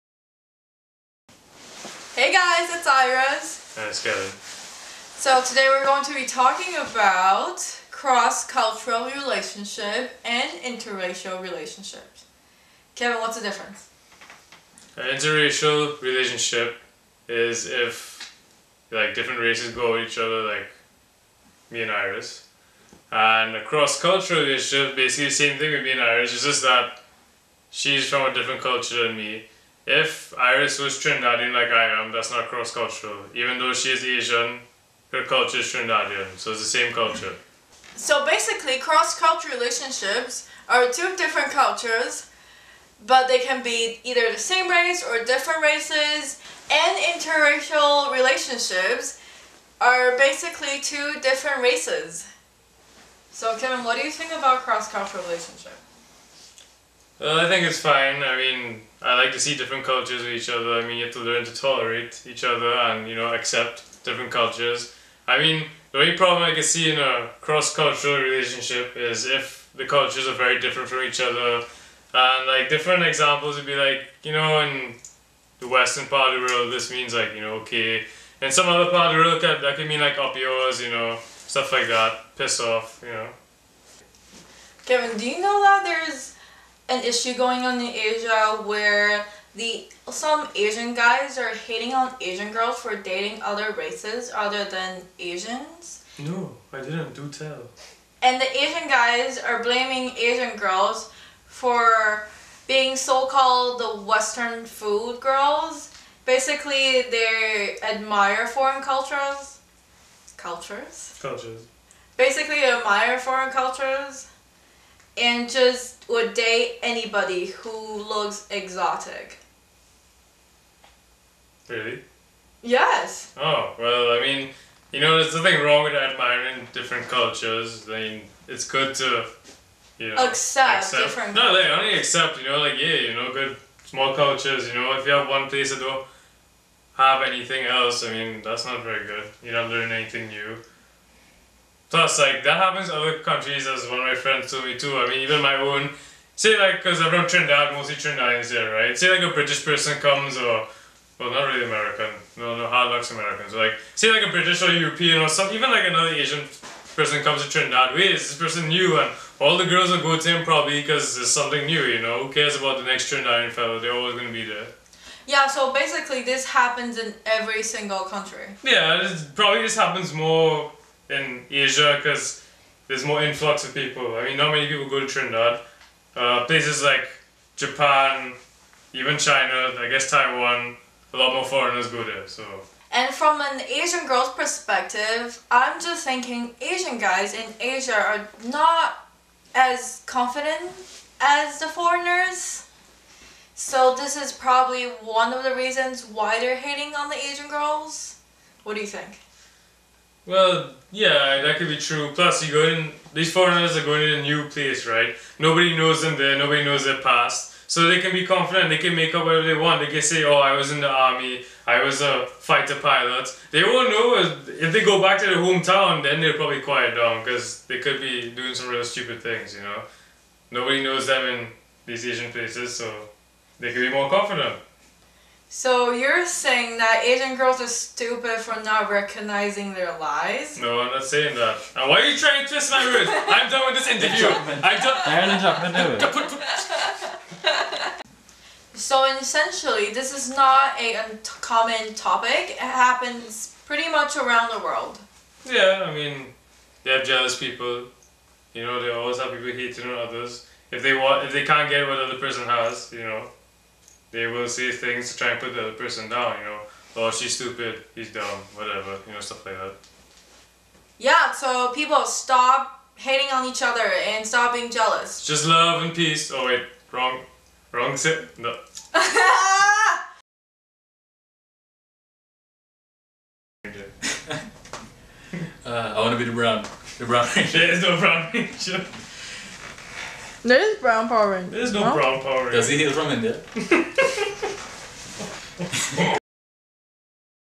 What You Don't Know About CROSS CULTURAL RELATIONSHIPS 關於異國戀
First requested video up! In the mist of increasing immigration resulting in diversity, more and more issues are rising between cross cultural relationships (CCR) and inter-rational relationships (IRR). This is a video talking about one of them which is the hate towards Asian girls from the Asian guys in Asia particularly.

Well, we tried to be neutral and unbiased. So if you are in any way offended, we apologize.

This vlog is filmed with HTC Re and edited via Adobe Premiere Pro, find out more about the camera on my review

Gain an Insight Into My life 🌈 @IrisViruss